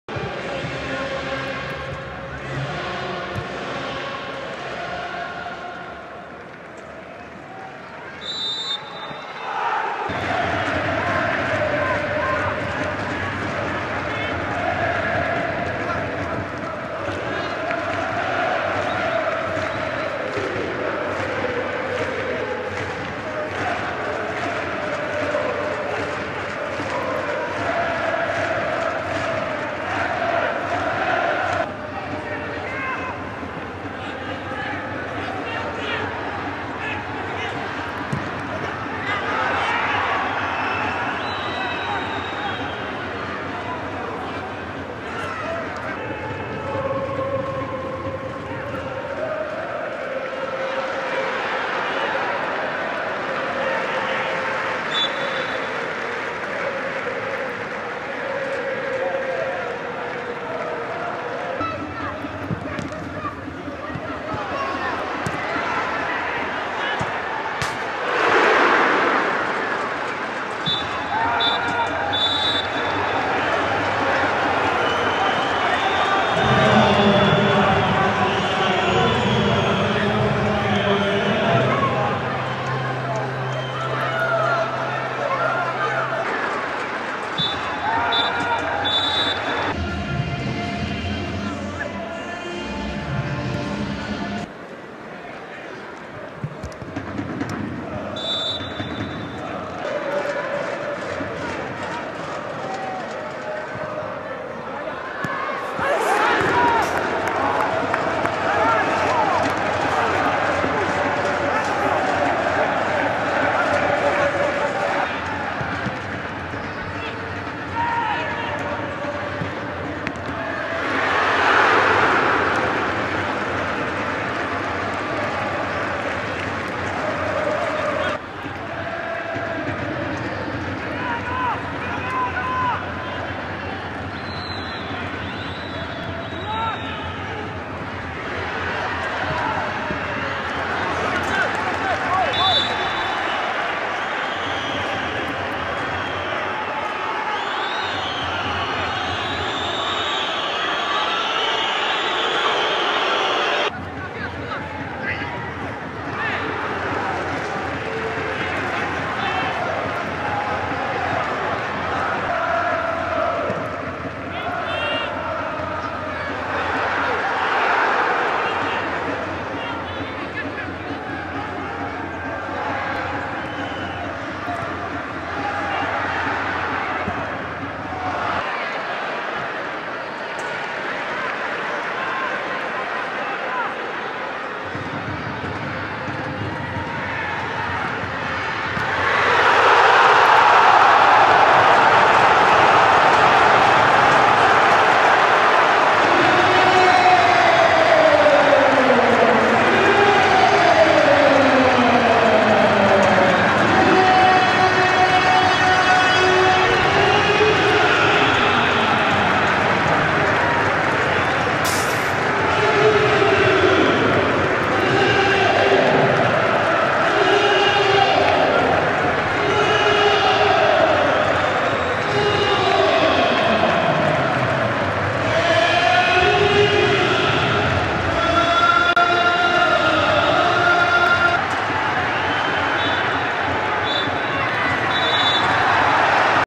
Highlights | Resumo: Boavista 1-0 P. Ferreira (Liga 17/18 #32)
Vitória por 1-0 do Boavista FC na recepção ao Paços de Ferreira, golo de Renato Santos a garantir os três pontos para os axadrezados, em jogo da 32.ª Jornada da Liga NOS.